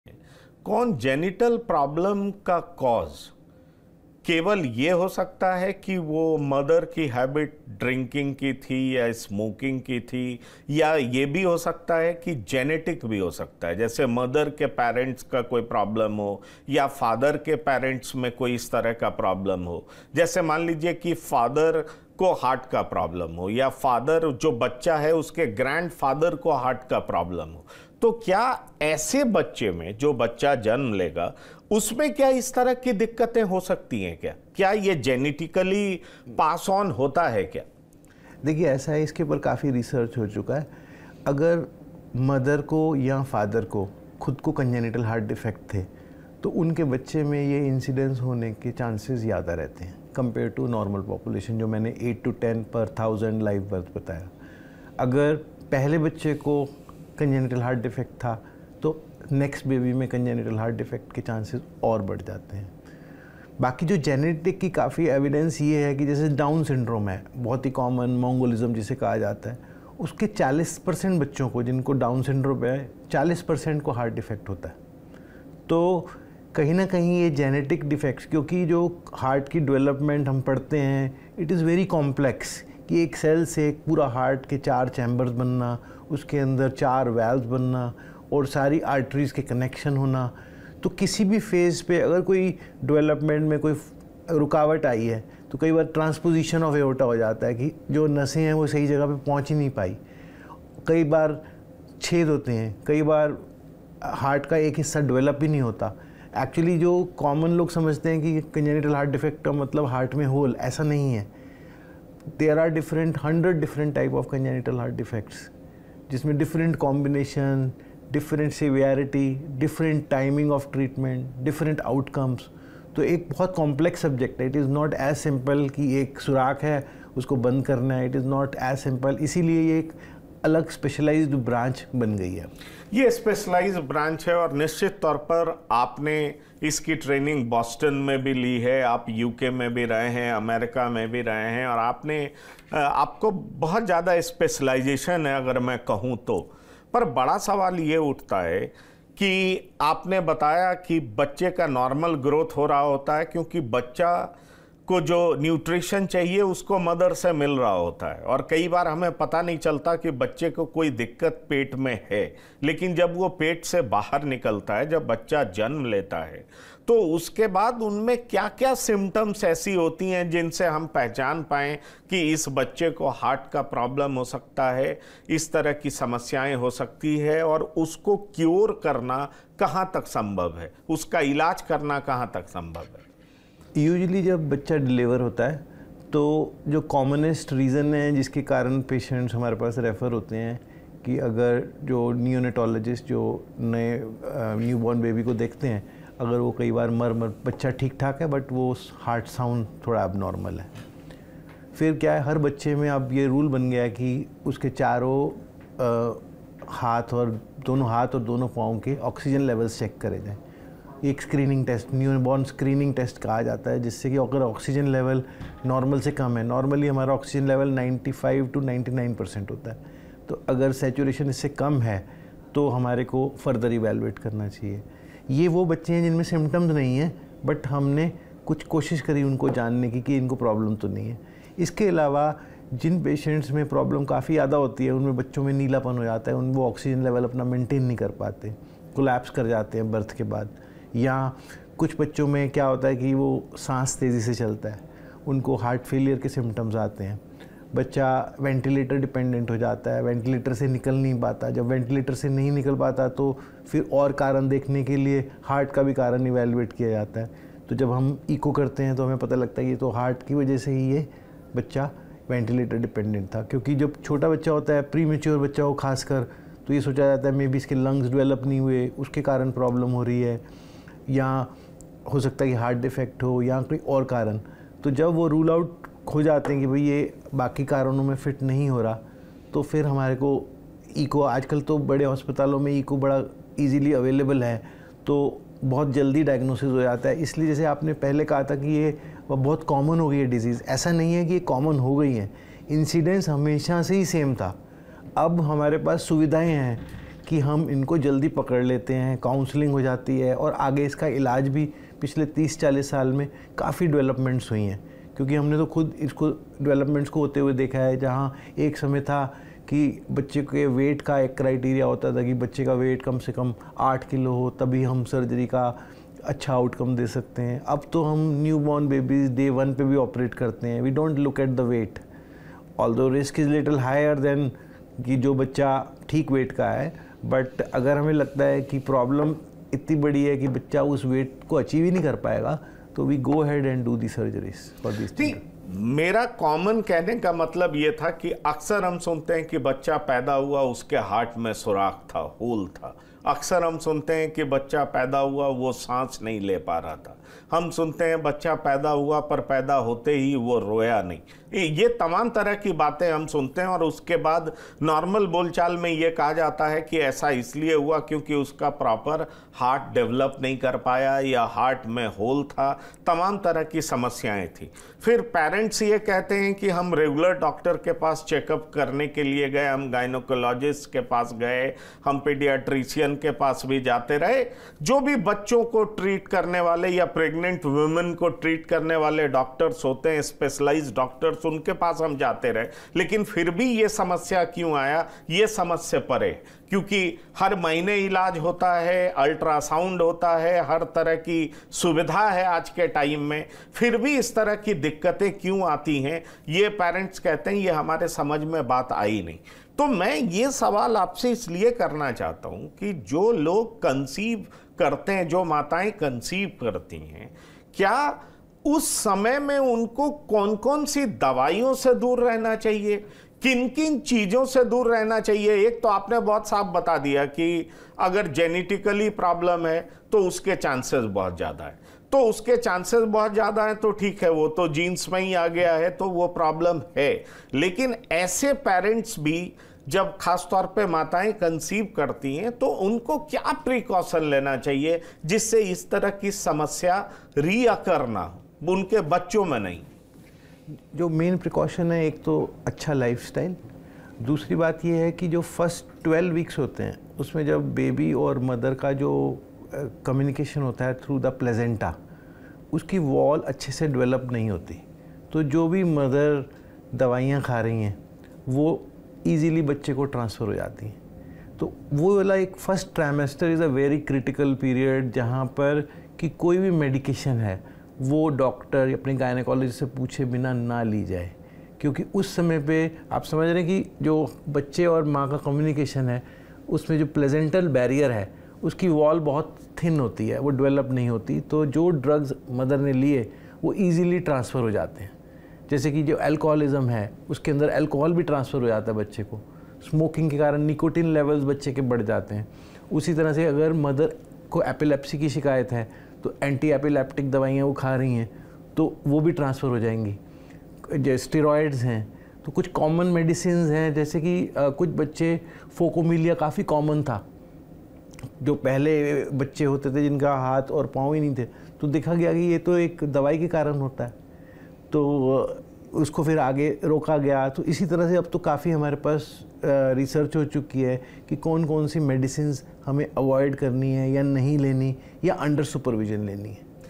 Dr Viresh Mahajan से जाने मां की हैबिट्स और परिवार की जेनेटिक समस्या भी बच्चों में आ जाती है ?
हमसे जुड़िए.